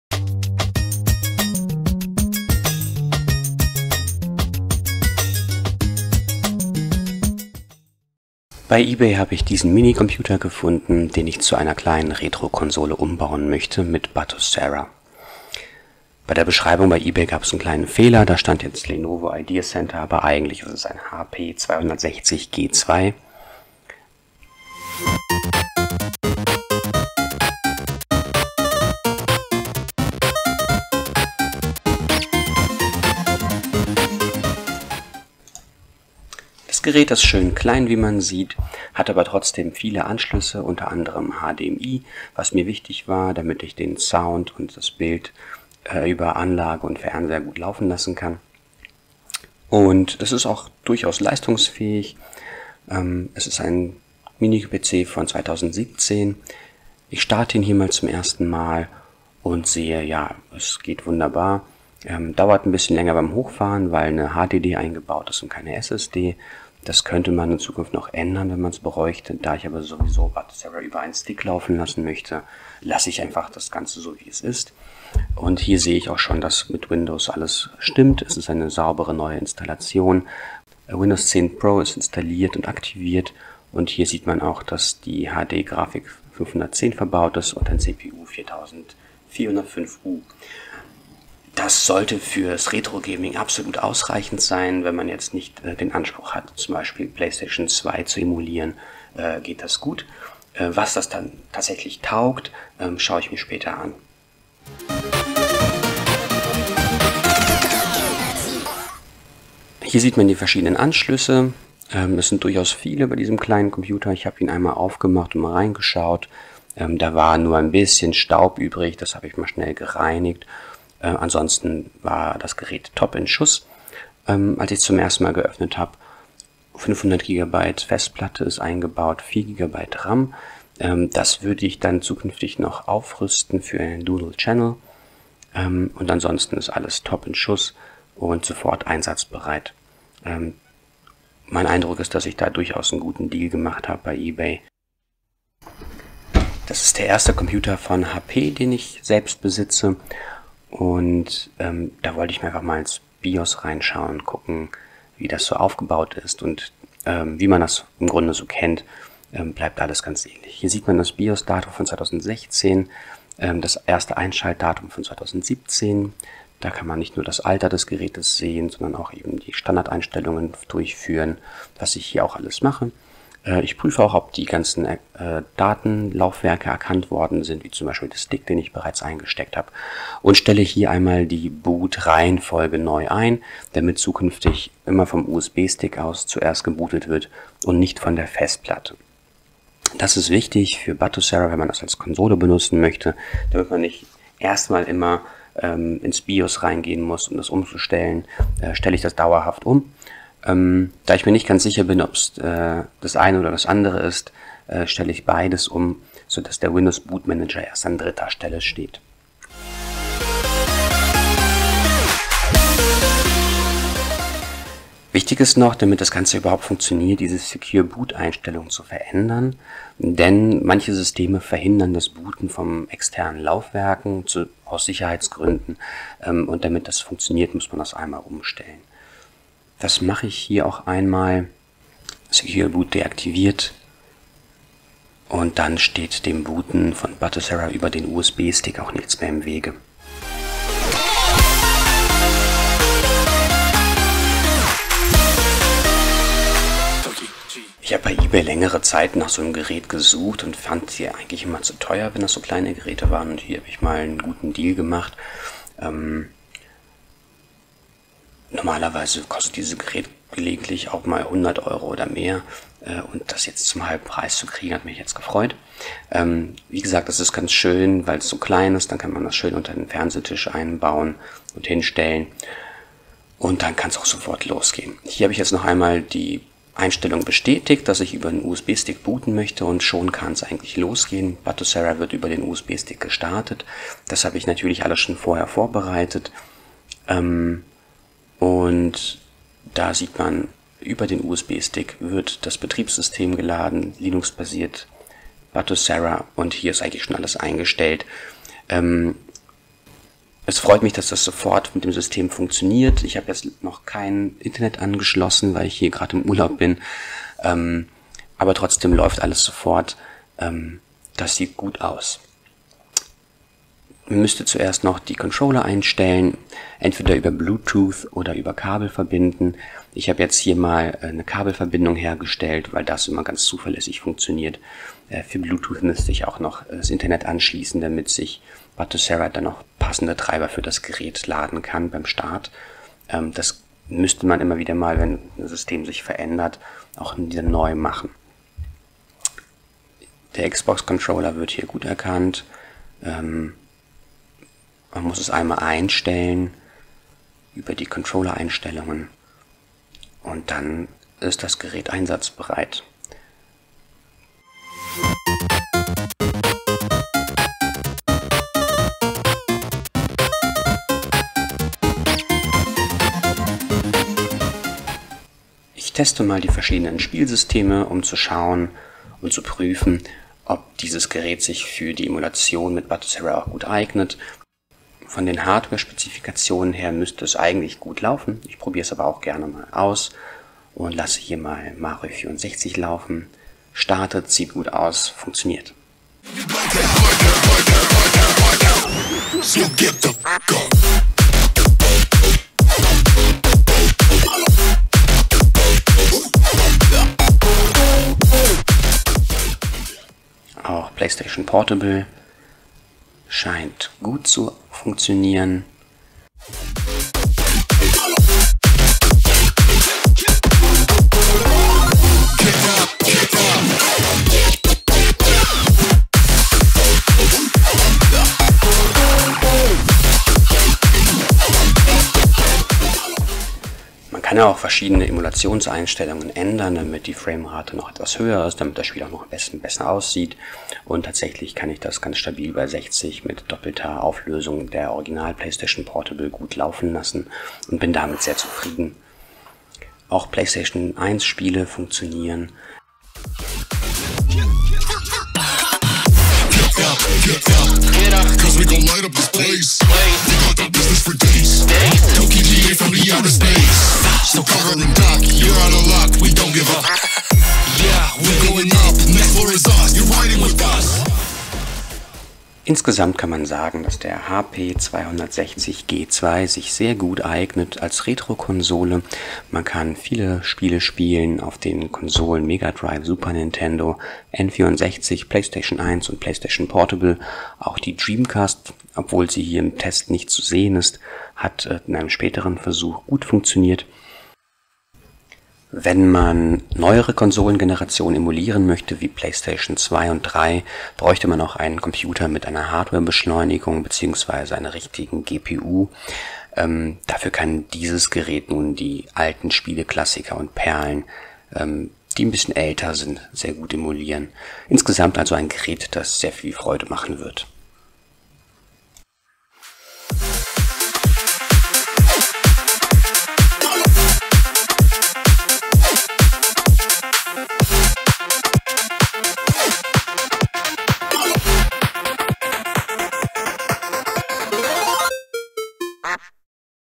Mein neuer Mini-PC fürs Retrogaming: Batocera auf dem HP 260 G2
In diesem Video starte ich zum ersten mal den HP 260 G2, den ich bei Ebay für 50 Euro ersteigert habe, und teste, wie gut der Mini-PC mit Batocera zurechtkommt.
Man sieht, wie man die BIOS-Einstellungen verändern muss,
um ein dauerhaftes Booten vom USB-Stick einzurichten.
Anschließend starte ich mehrere Spielemulationen z.B. PSP und PSX.

iPhone SE
Movie Maker 2012
Resolve 16